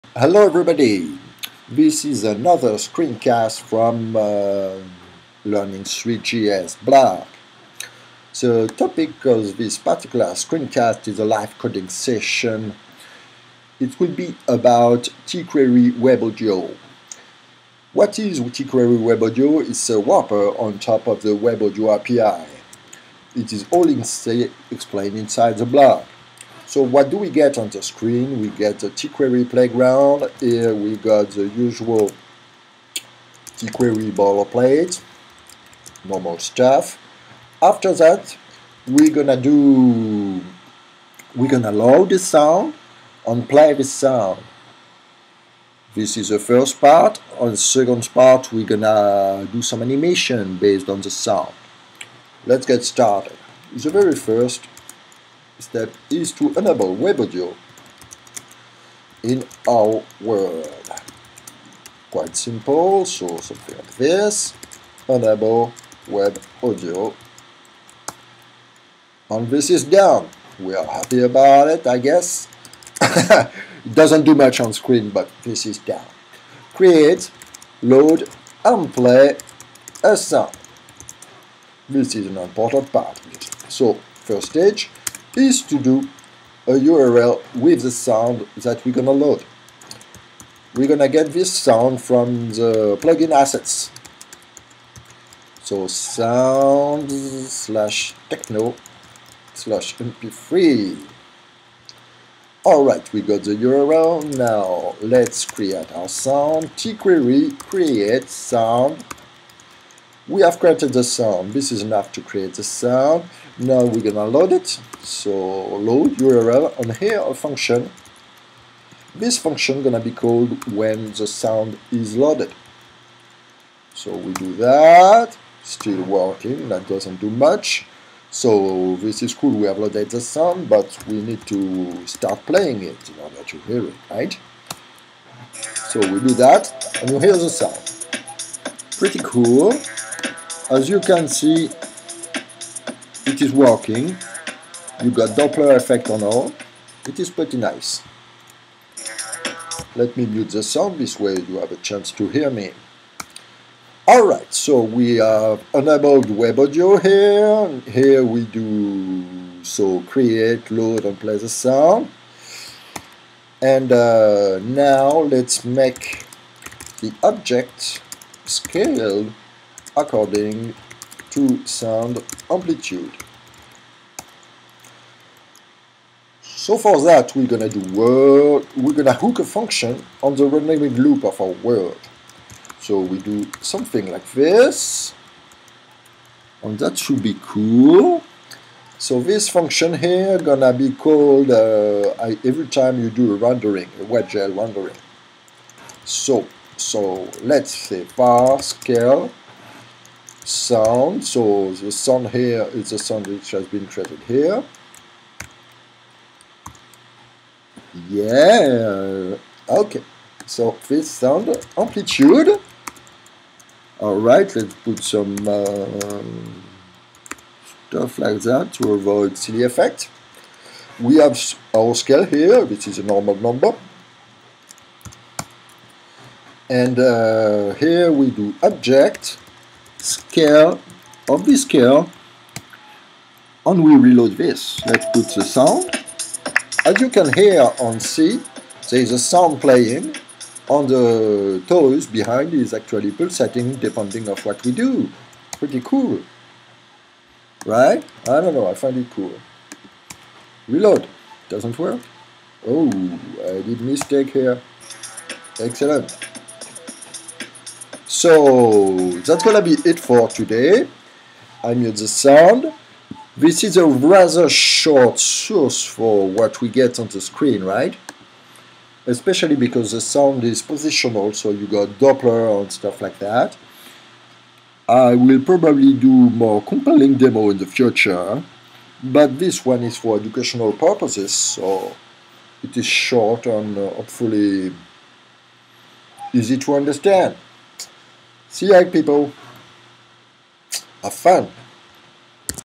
tQuery WebAudio for more realistic 3D
blog. It shows a short live coding session to load, play a sound with tQuery.webaudio.